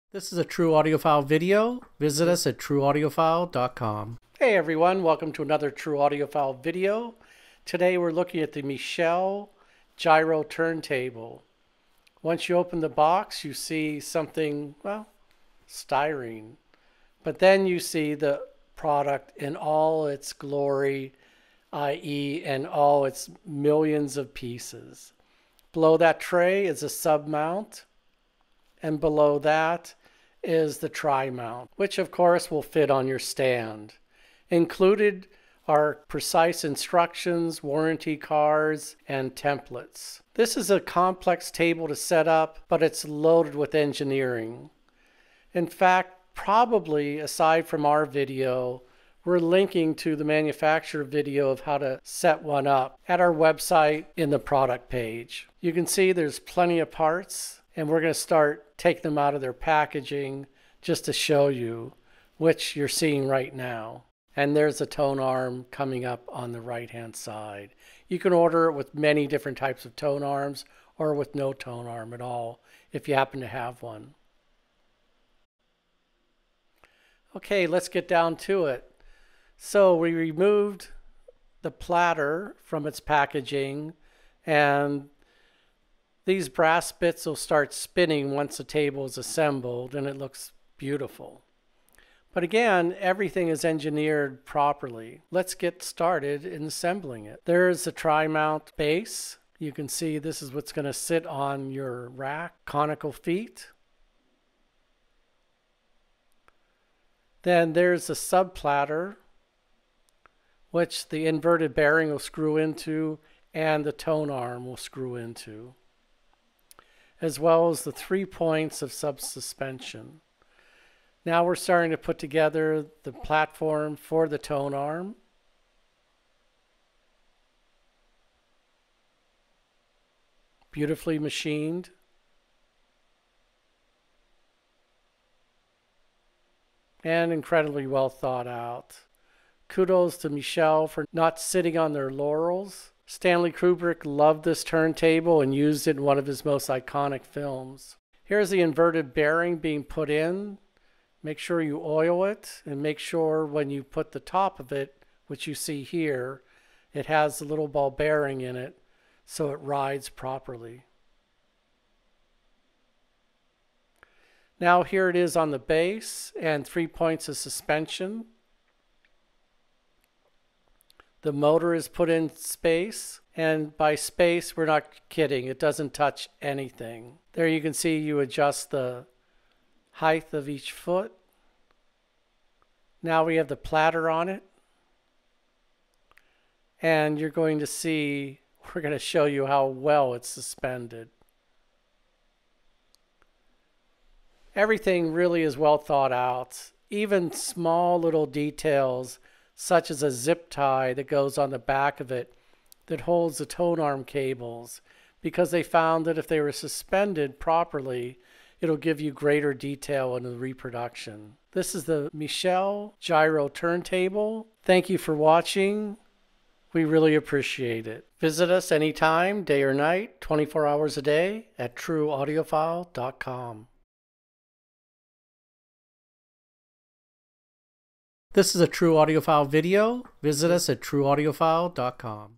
Michell Gyro TT True Audiophile Mini Review & Unboxing. Michell gorgeous Gyro Turntable
The Michell Engineering Gyro SE Turntable. An Icon. A work of art. A sonic delight at its price.

AWARD WINNER PRODUCT OF THE YEAR 2022 : To my eyes, the Michell Syro SE is a thing of beauty in every way—from material choice, to design, to build quality which strikes me as fairly astounding given its price. I will be talking about how it sounds in the not too distant future. Right now, I find myself just looking throughout the day to see how the changing light makes a movie in shadows.

True Audiophile is dedicated to providing people with music playback that is as stunning as it is an incredible value at every price point.